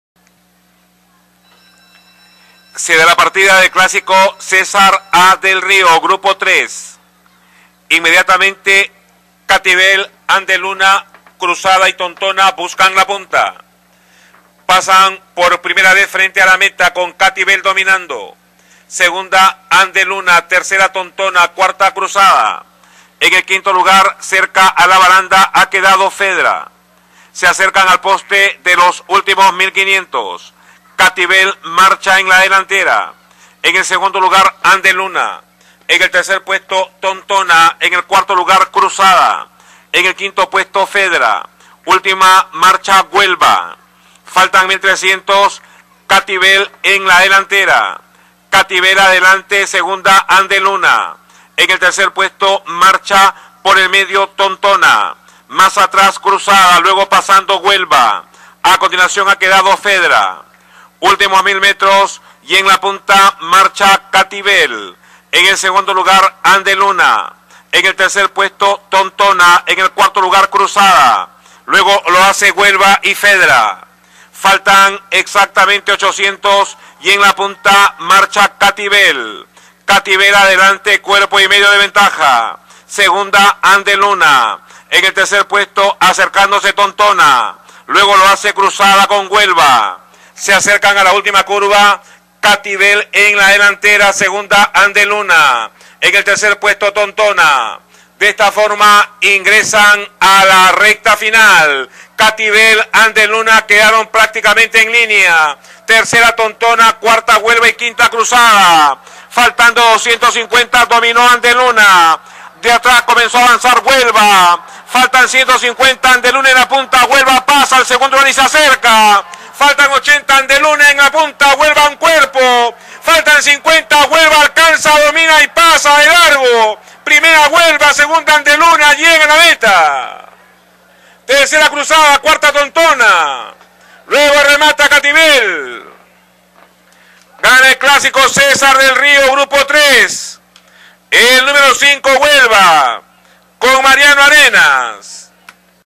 HUELVA - Clasico CESAR A. DEL RIO 2012
1º  5  Huelva (Kela)
2º  2  Andeluna (Mineshaft)  1 Cpo.
3º  6  Cruzada (Quintillón)  3½ Cuerpos
4º  4  Tontona (Powerscourt)  4 Cuerpos
5º  3  Catibel (Flanders Fields)  10¼ Cuerpos
6º  1  Fedra (Farallon P.)  11½ Cuerpos

2000m Tiempo: 2.07 2/5

Huelva es una castaña de 6 años, por Kela y Thunder Falls (Thunder Gulch), nacida en USA y que pertenece al Stud Quaker State. Andrés Arredondo presentó en buena forma a la conducida por Mariano Arenas, quien estuvo impecable en sus controles. Esta fue la 8va. victoria en 40 salidas para Huelva que elevó sus ganancias a S/. 164,457.